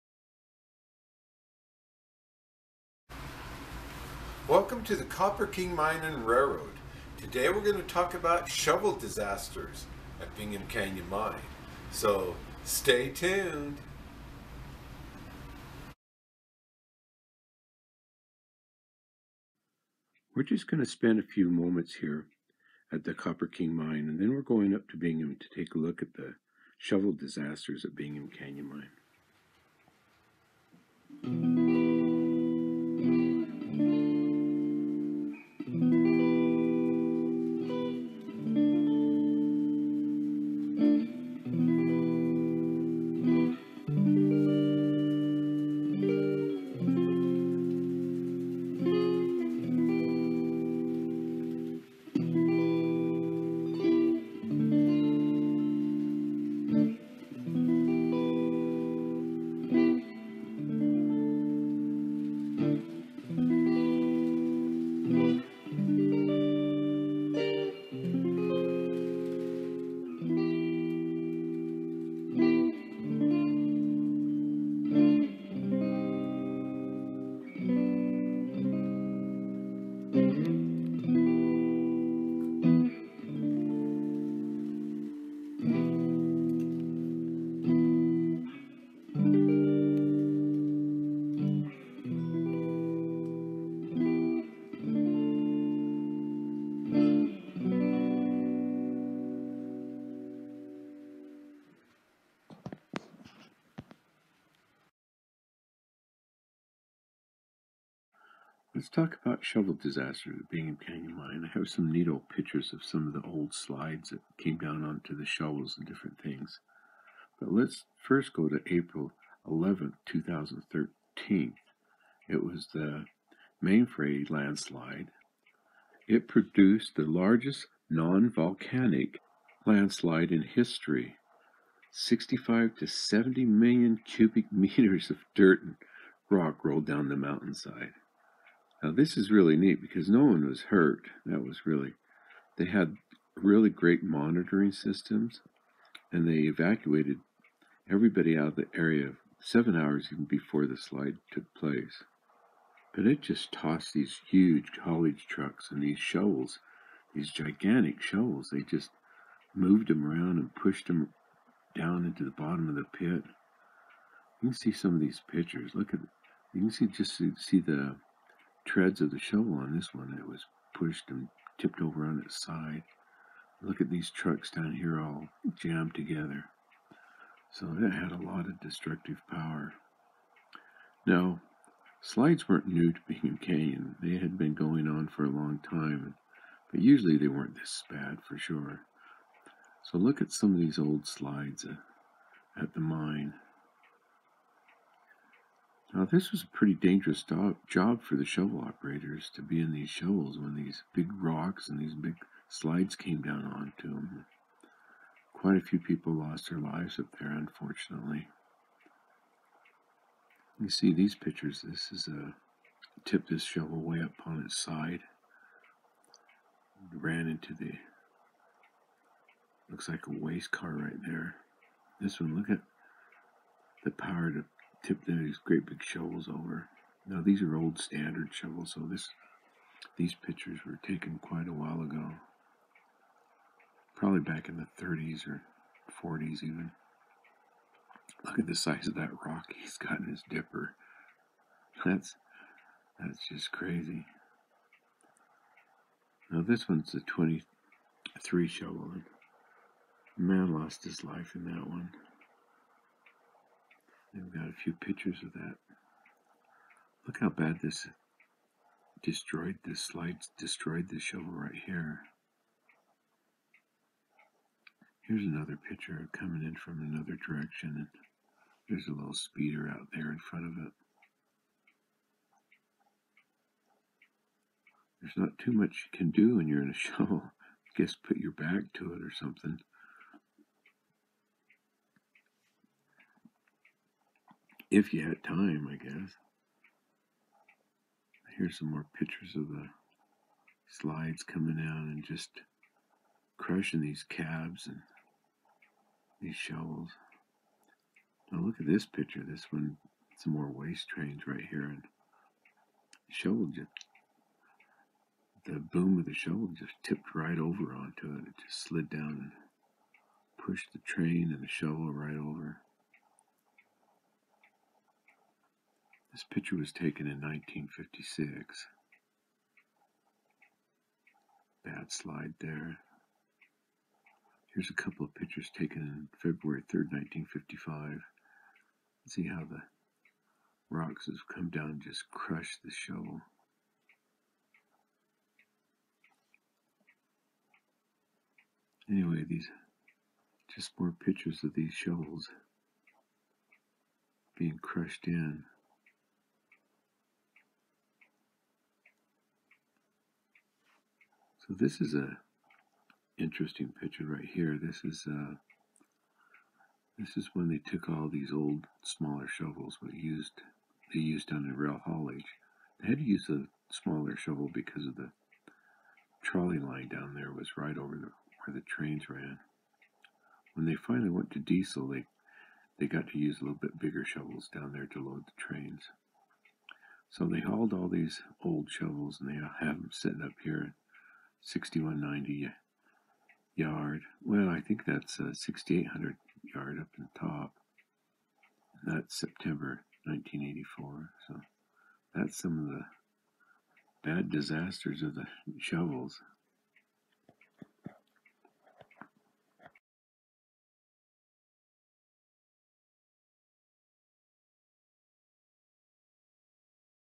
44. Shovel Disasters at Bingham Canyon Mine.
Buy my new book Bingham Canyon, help keep Bingham’s history alive. Slides coming down on the large electric shovels. Not a very safe job when you are a shovel operator. Manefay slide April 10, 2013 was the largest nonvolcanic landslide in history. Pictures from Don Strack (utahrails.net) and Larry Sax collection (with permission), and the Utah Historical Society.
Corrections, the old standard shovel tipped its side was caused by overloading the dipper with a large boulder. The shovels lineup at the end of the video are at Yosemite shop area.
Link, 23. Shovels, for loading ore and waste cars. - YouTube